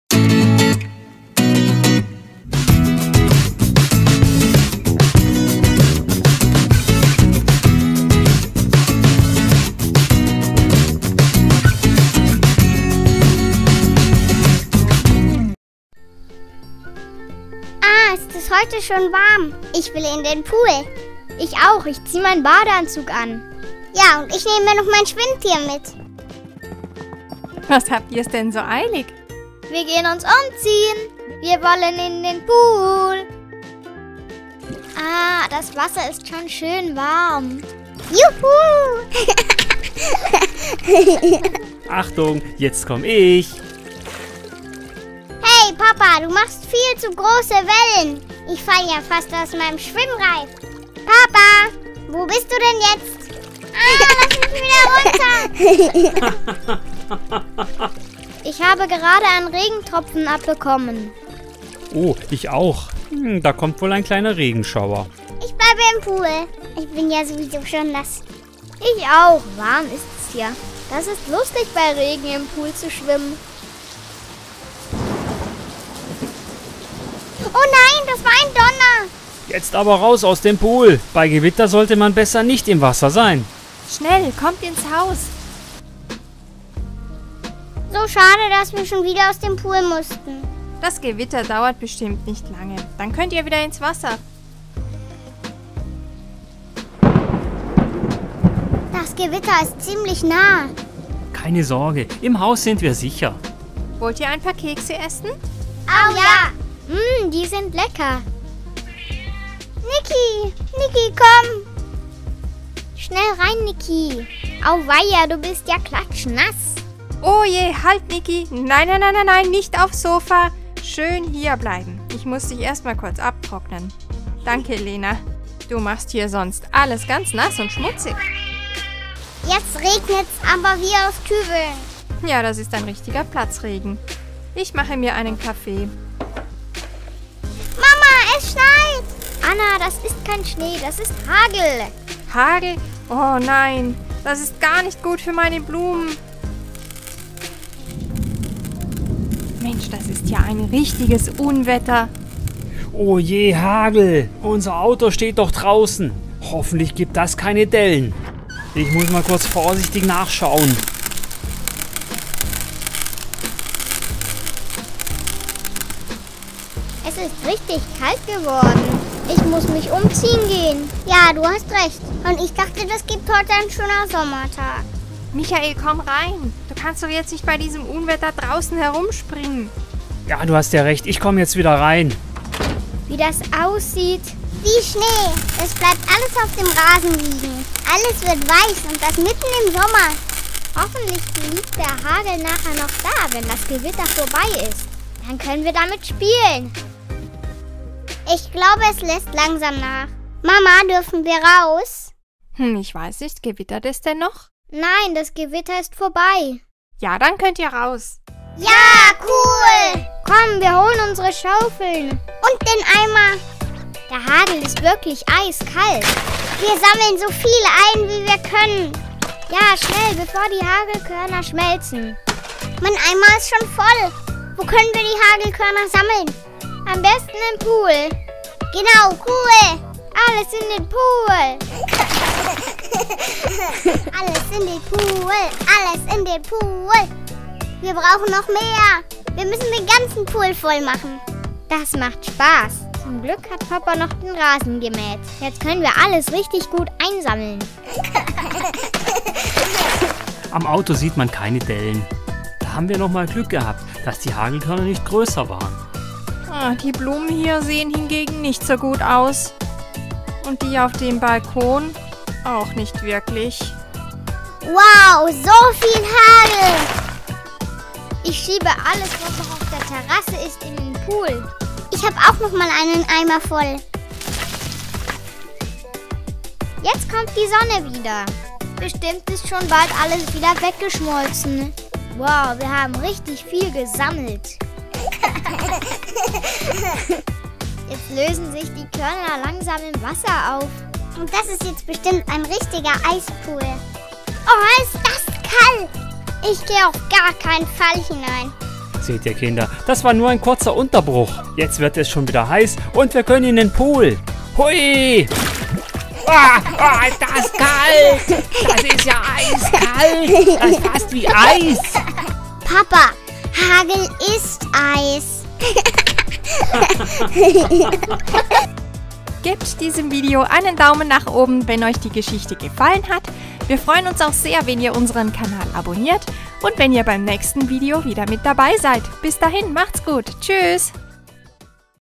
Playmobil Familie Hauser im Hagel - Pool Geschichte mit Anna und Lena
PLAYMOBIL Familie Hauser genießt den Sommer im Garten und im Pool. Doch plötzlich kommt ein Unwetter... Viel Spaß!

Auf unserem Kinderkanal Familie Hauser seht ihr tolle Kinderfilme.